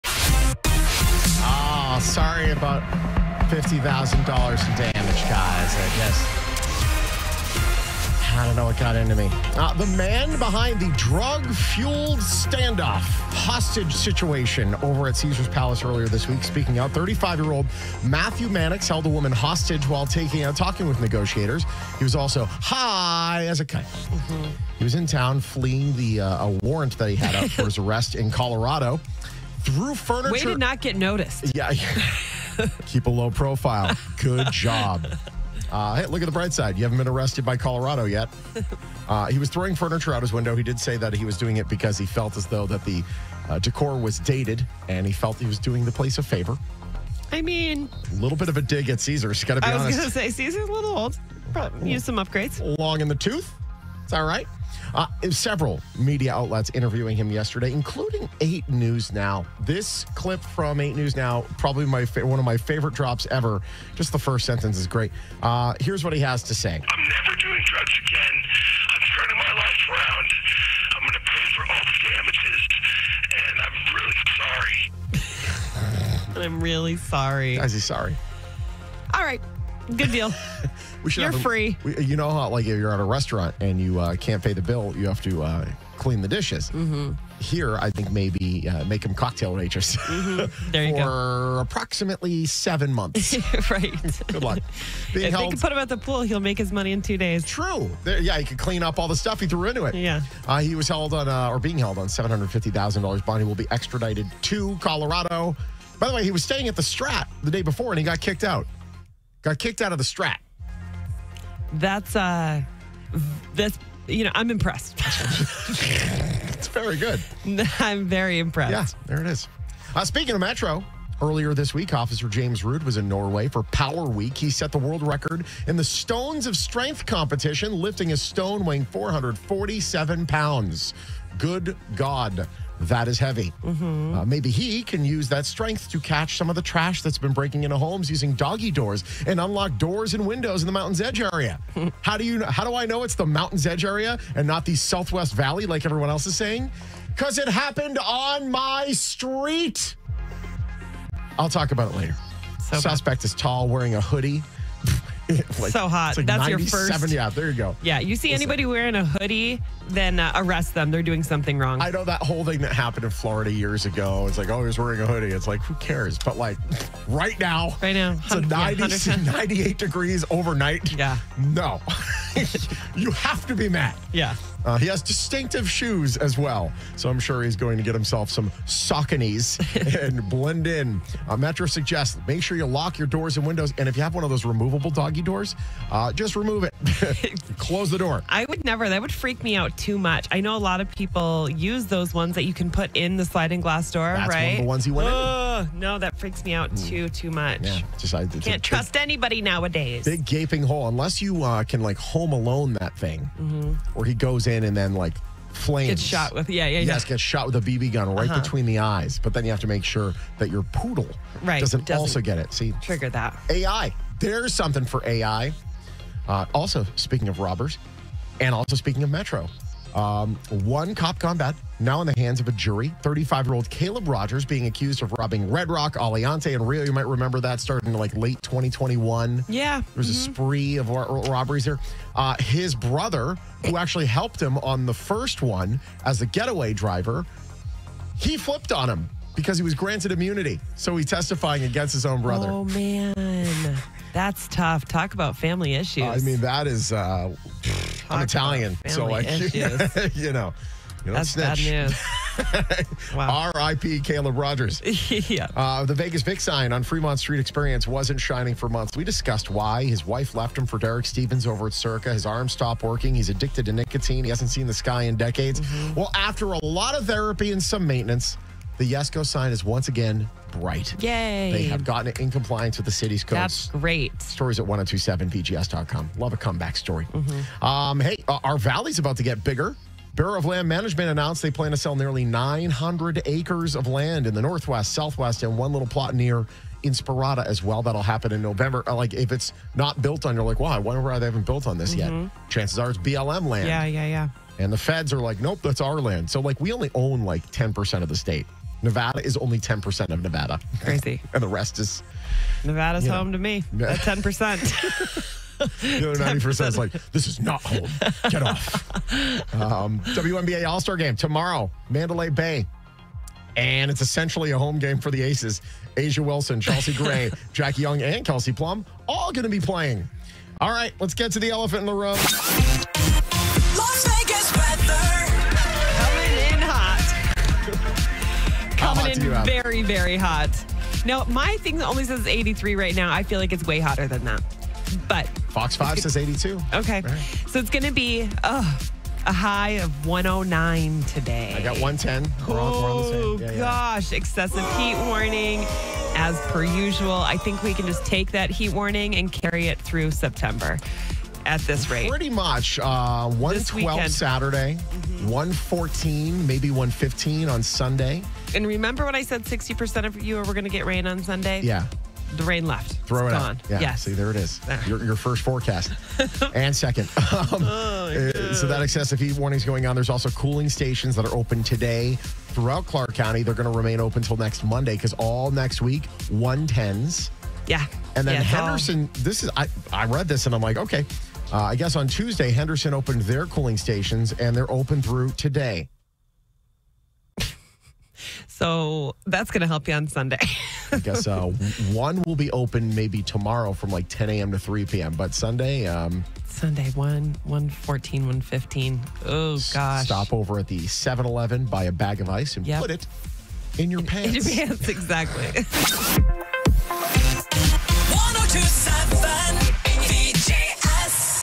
Need To Know: Caesars Palace Las Vegas Hostage Taker Is Sorry, You Guys
He held a woman hostage for hours. He caused $50,000 in damage. But the Caesars Palace Las Vegas hostage taker guy says he is sorry and he wants to make amends.

Plus someone is sneaking into doggie doors in the southwest valley.

📷 Khosrork via iStock/Getty Images Plus